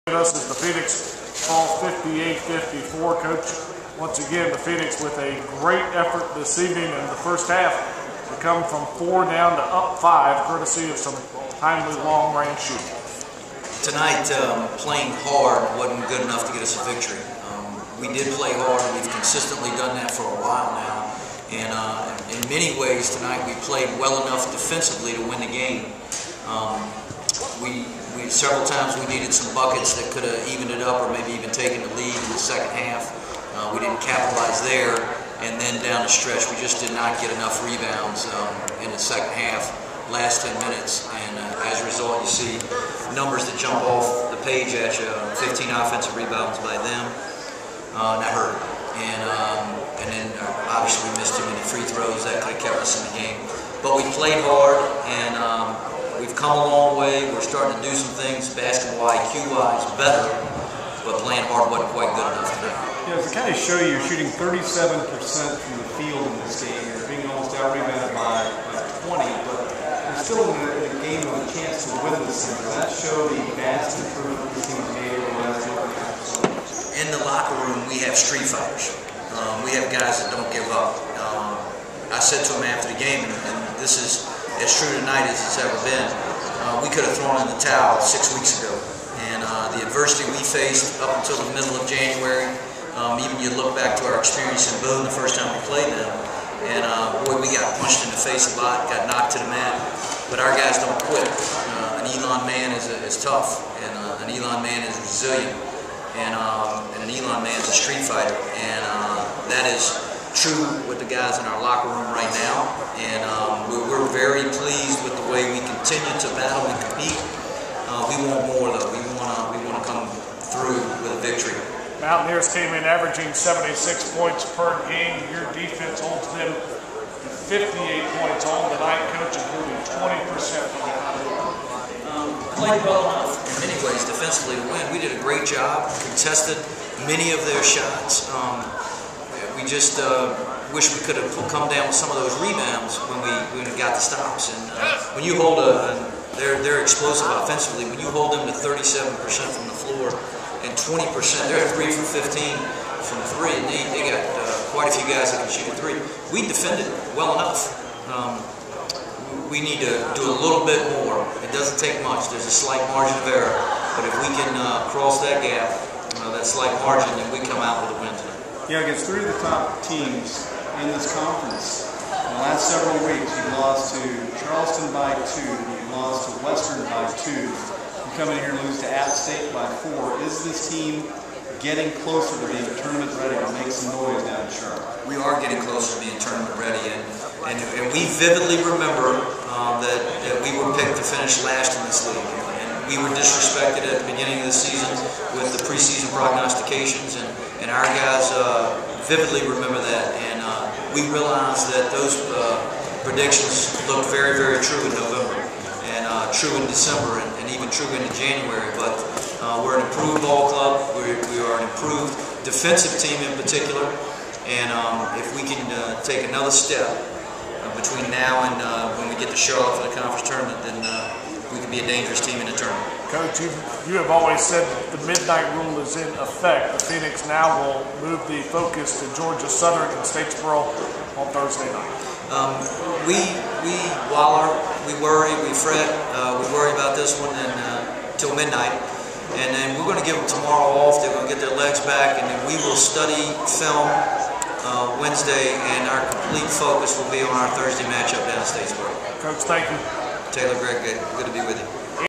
Coach Matheny - Appalachian State Postgame
Hear head coach Matt Matheny's thoughts on Elon's 58-54 loss to Appalachian State in Alumni Gym.